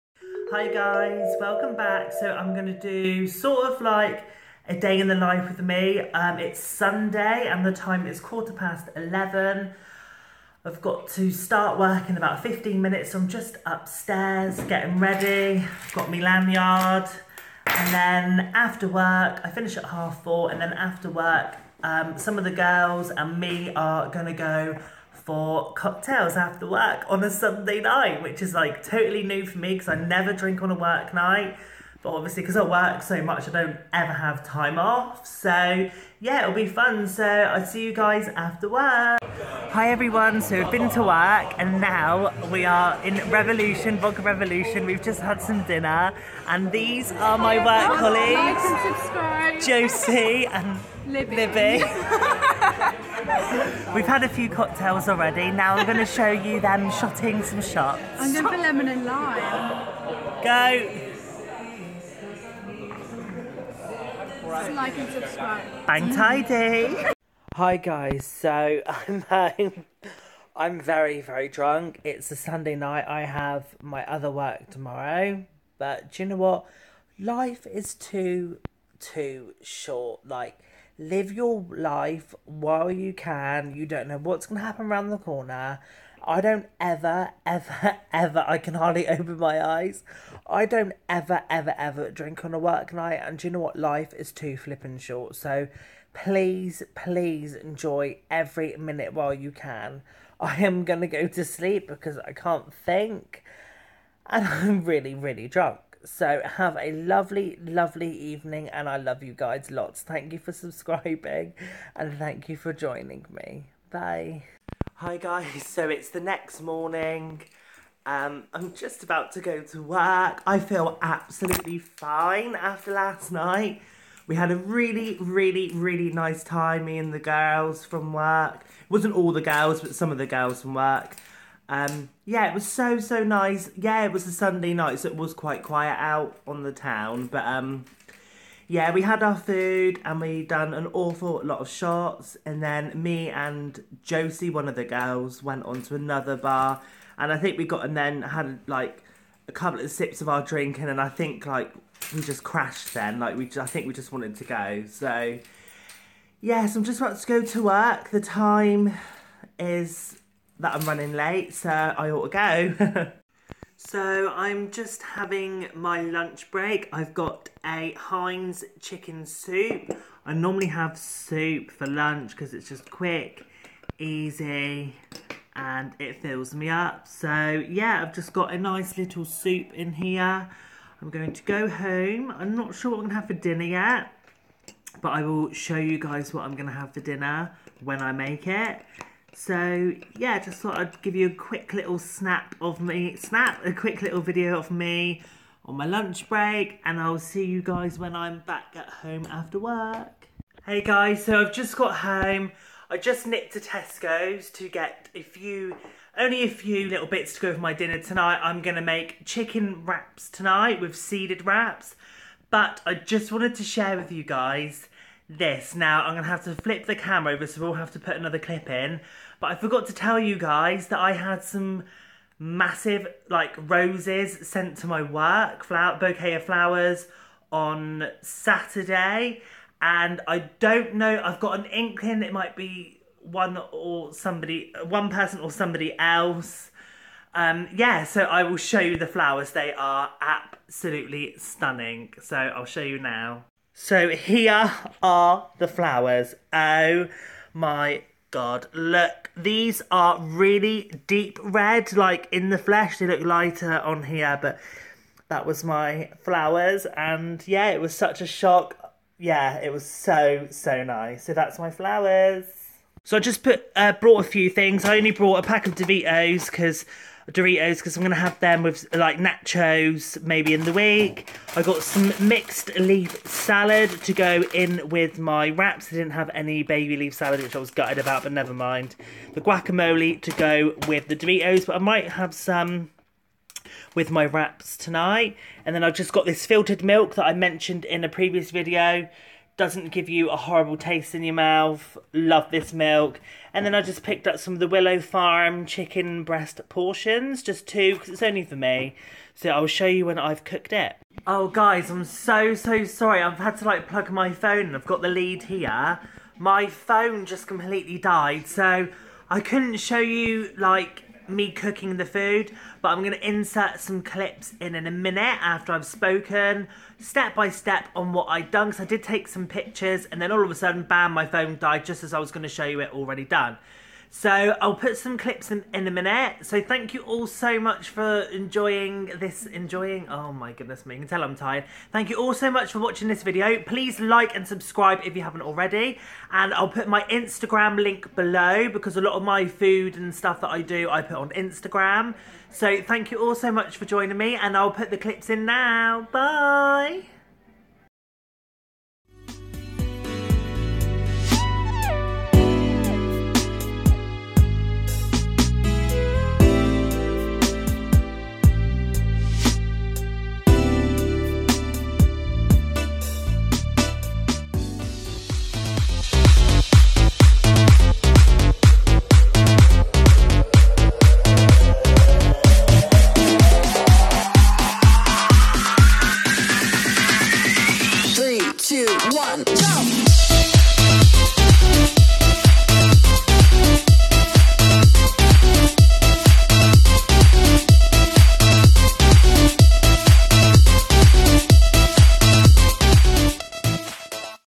DAILY DAY IN THE LIFE VLOG | DRINKS WITH THE GIRLS | DRUNK | FOOD
Day In The Life Vlog (Drinks With Some Of The Work Girls)

SORRY FOR THE DRUNKEN CLIPS LOL!!!